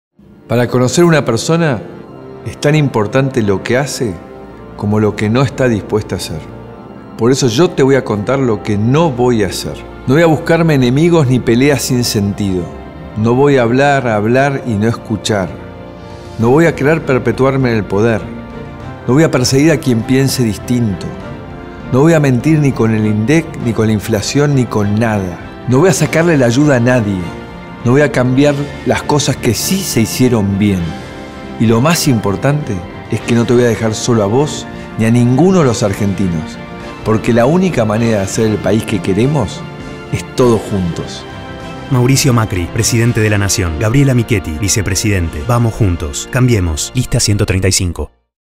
Lo que no voy a hacer
Mauricio Macri, Spots de campaña presidencial, Argentina, 2015